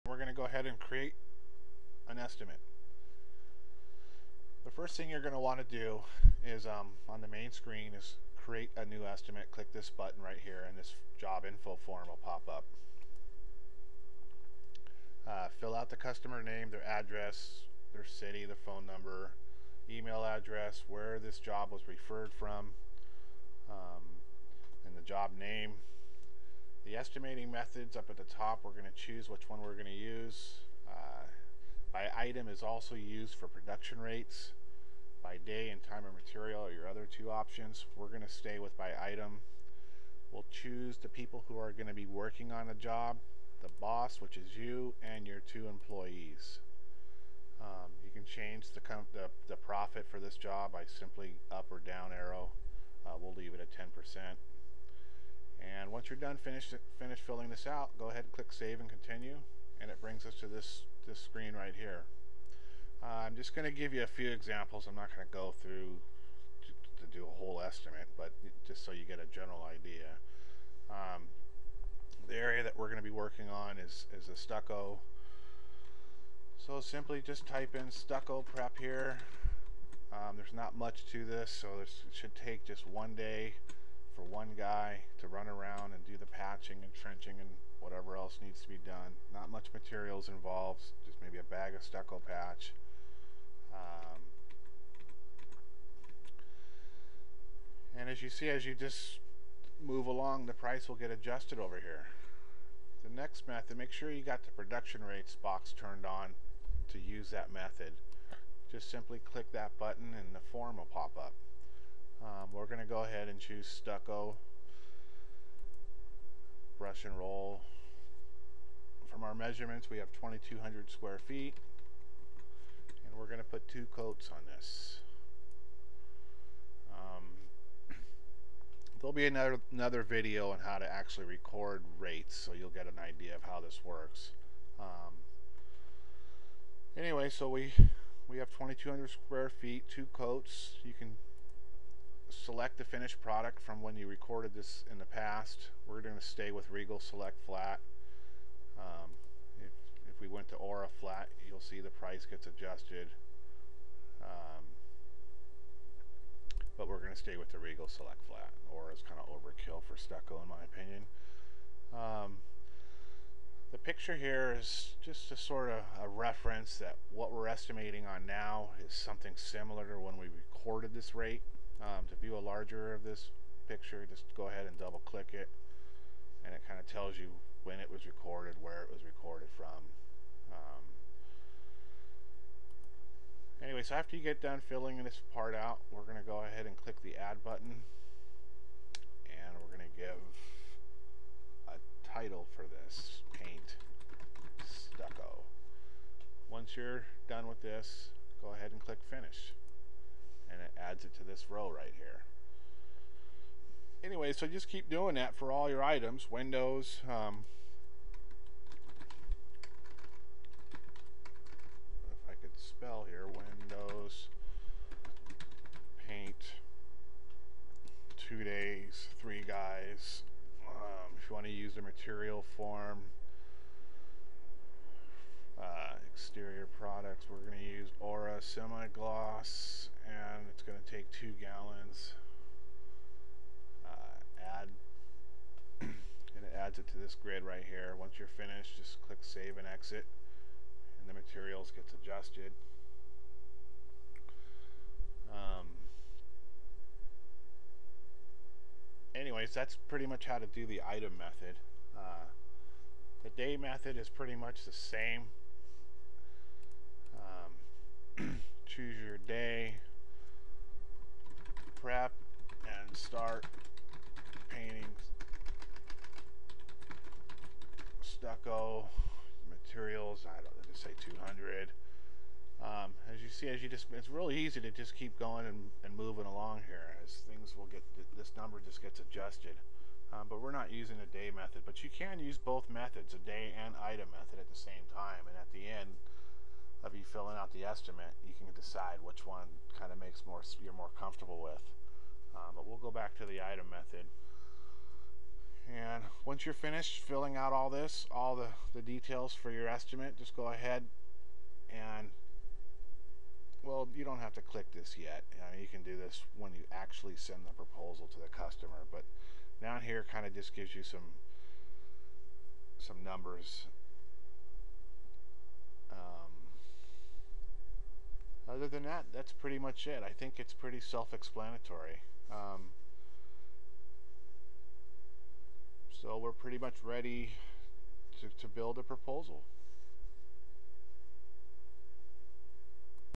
Software for painting contractors - Creating a Estimate using The Paint Estimator
Estimating software for painting contractors
How to create an estimate using The Paint Estimator